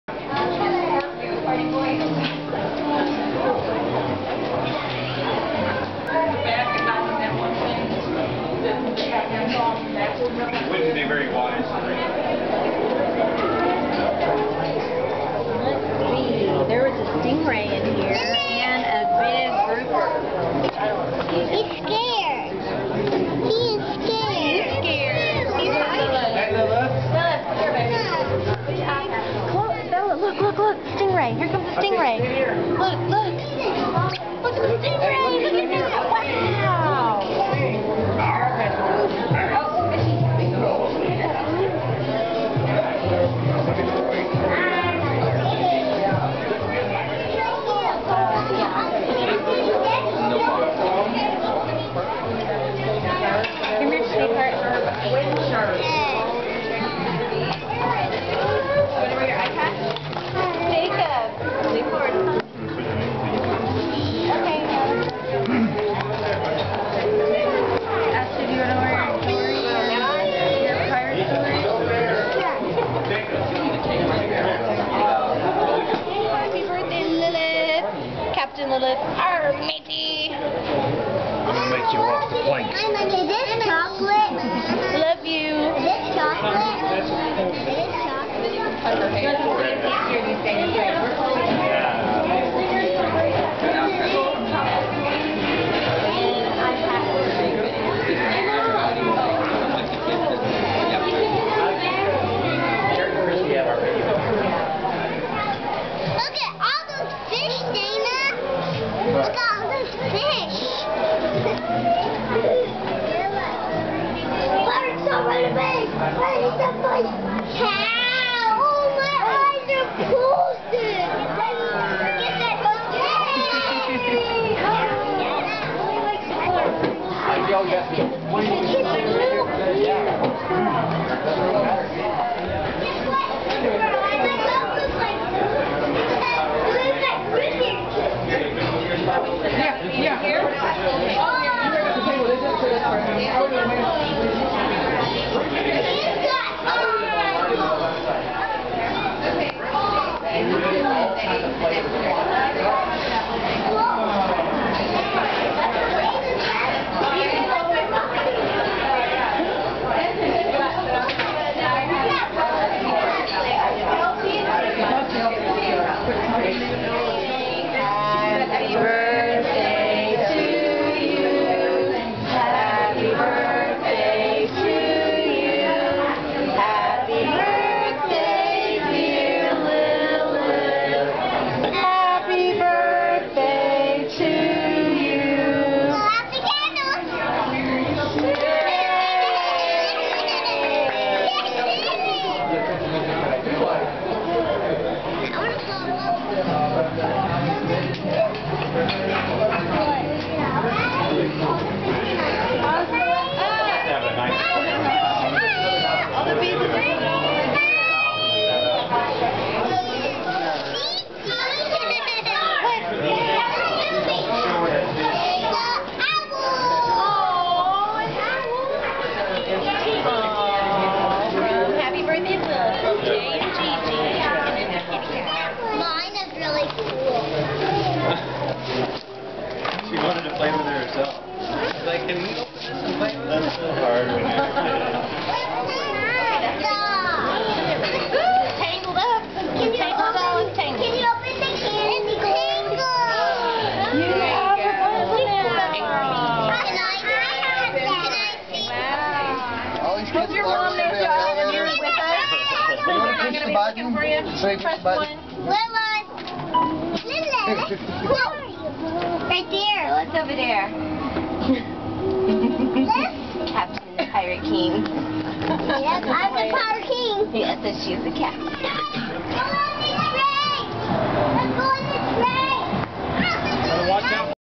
Liliths Birthday pt 1.avi
Lilith's Birthday Party - part one. Dining, Birthday cake & Presents!